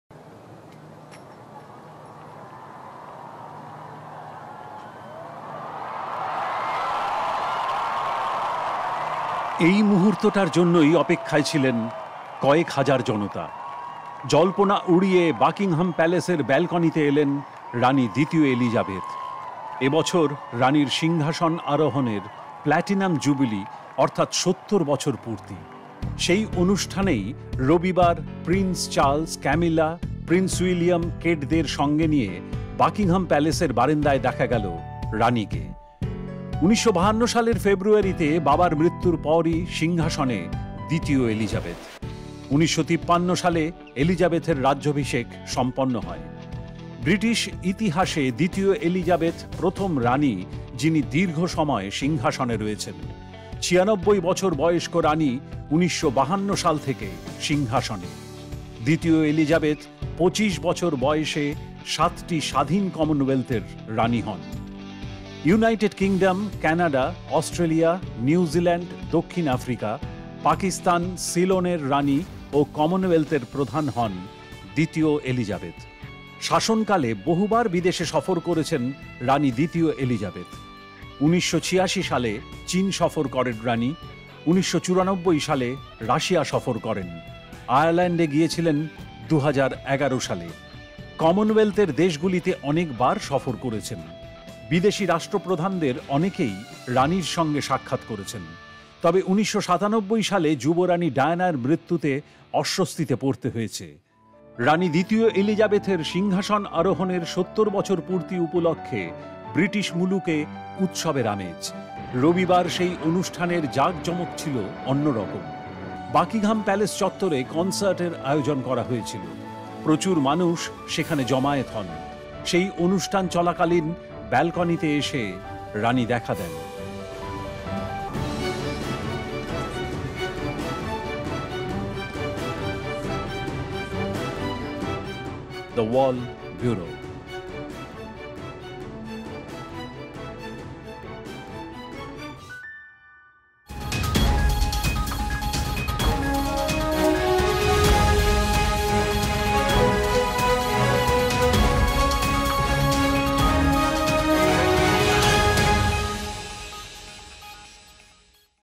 রানির শাসনে ৭০ বছর, ব্যালকোনিতে এসে দাঁড়ালেন রানি দ্বিতীয় এলিজাবেথ
⭐ Pujo Porikroma

⭐ Aparajeyo

⭐Pujo Parba